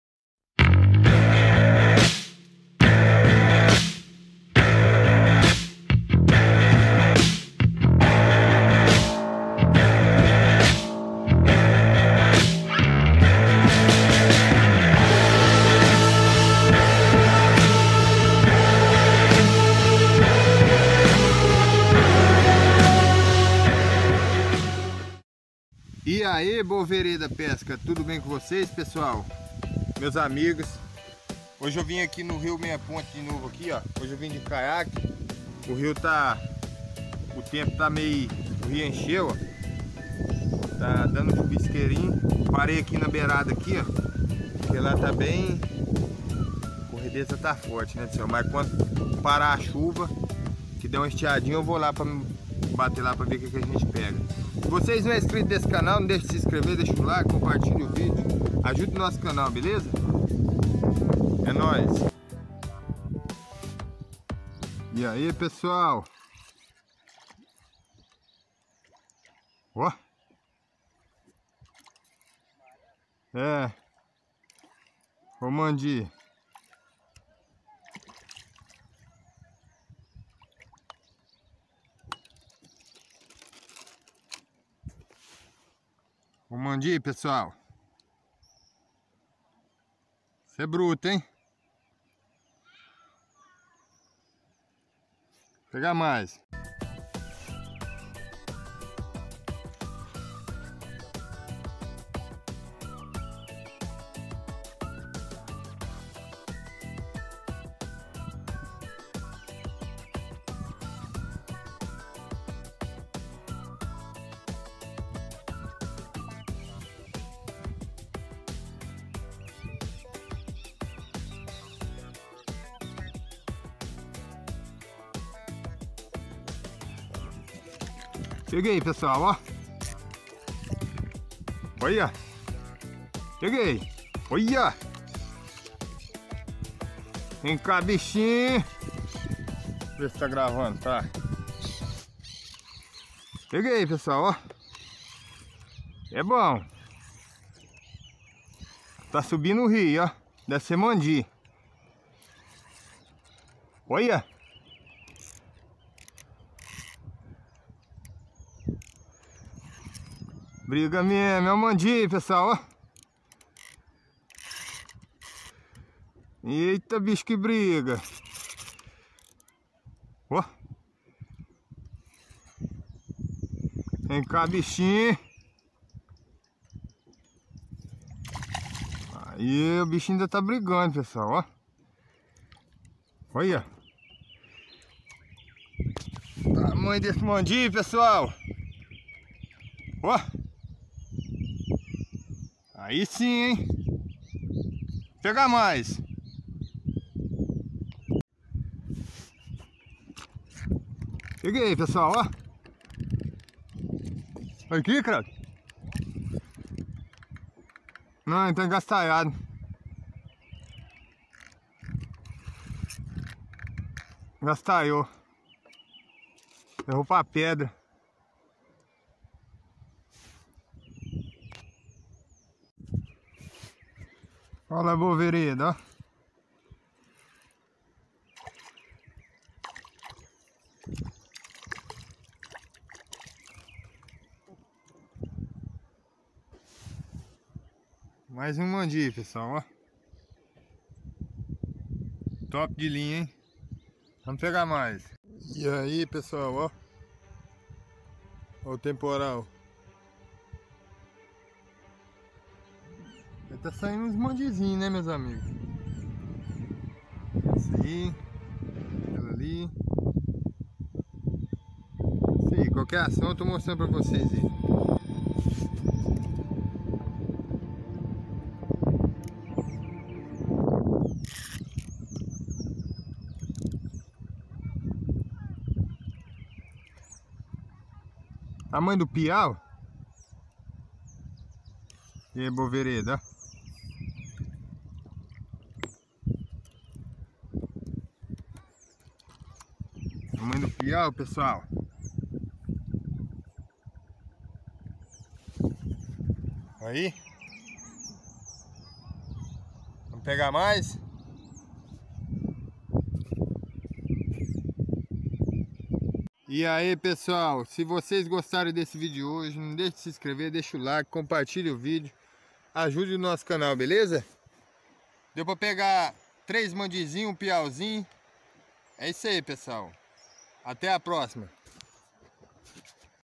Pescaria de mandi em dia chuvoso saiu bruto
Hoje fui fazer aquela pescaria no nosso rio meia Ponte amigos espero que vocês gostem